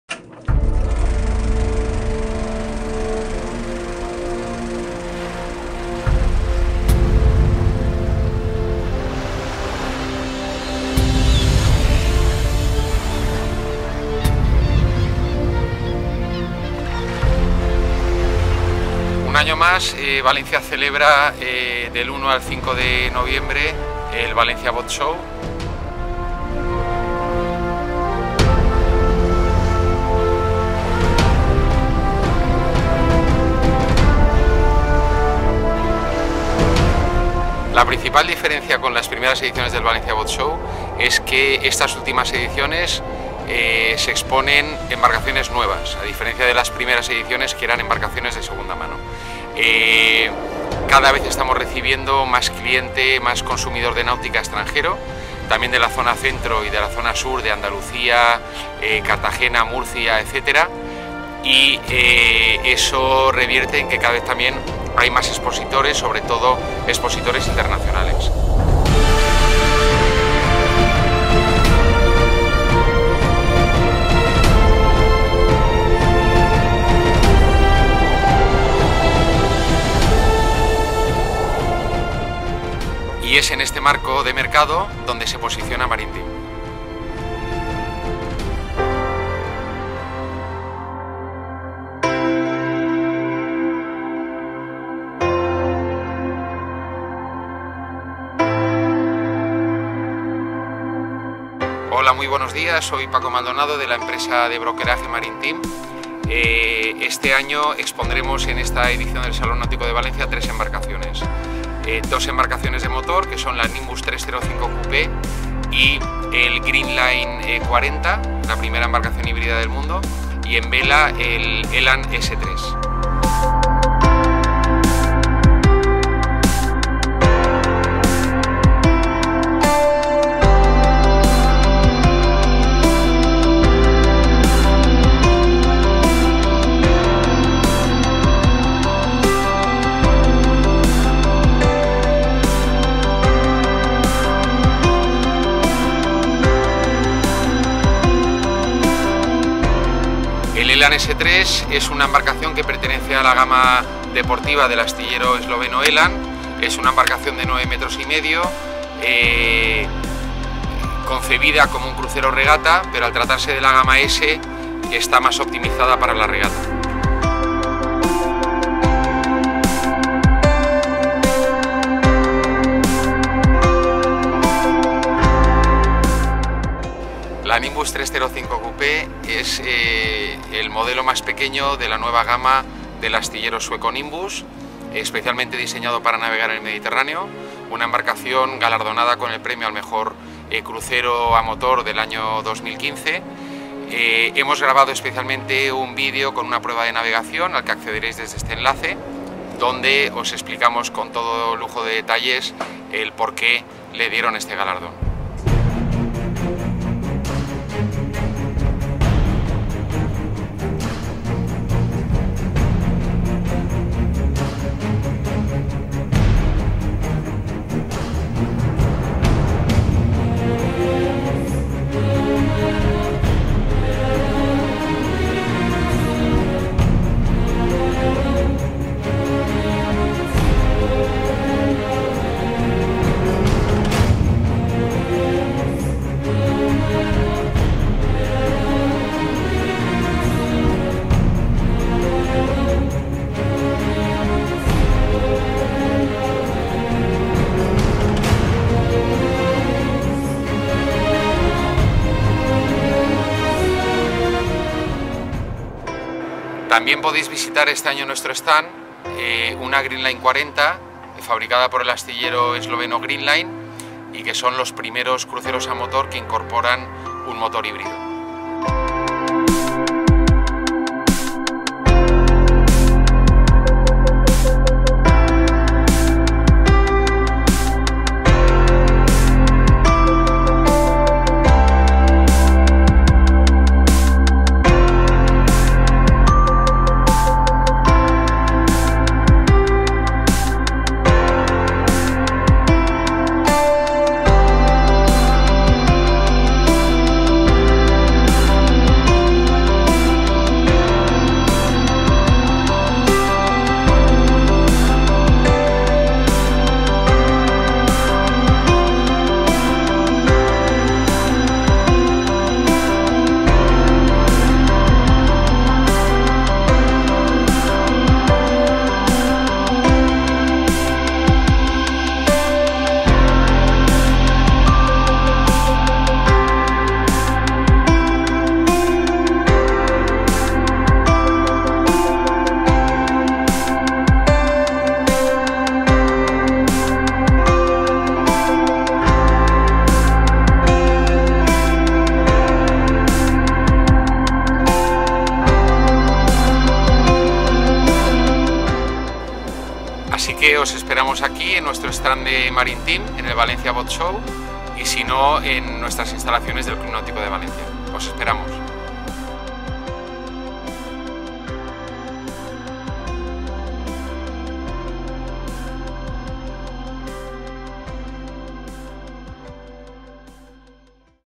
MARINE TEAM EN EL VALENCIA BOATSHOW 2017
Marine Team presenta su stand en el Valencia Boat Show 2017.